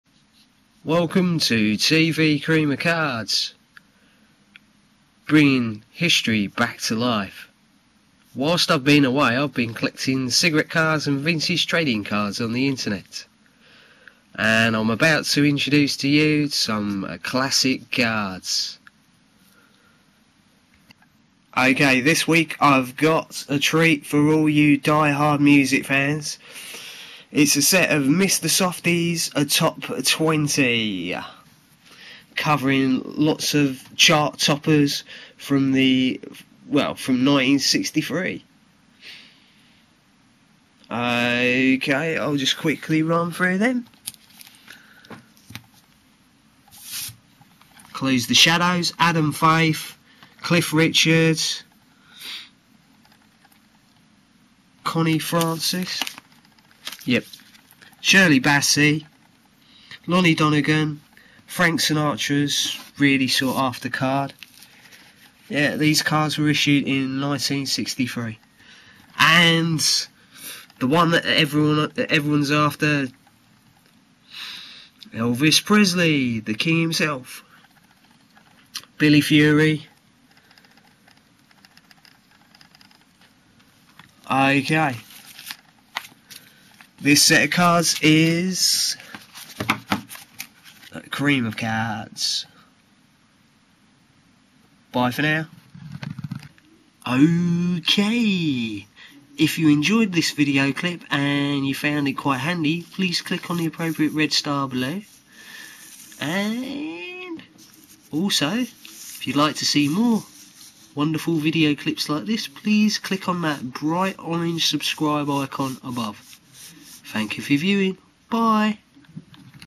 1960's Rock and Roll & Pop Stars Memorabilia + Elvis Presley
Elvis Presley introduces the following set of 1960's trading cards:

Manufacturer: Mister Softee
Series Title: Top 20
Year Of Issue: 1963
Card Size: normal
Complete Set: yes
Number of Cards Offered: 20 cards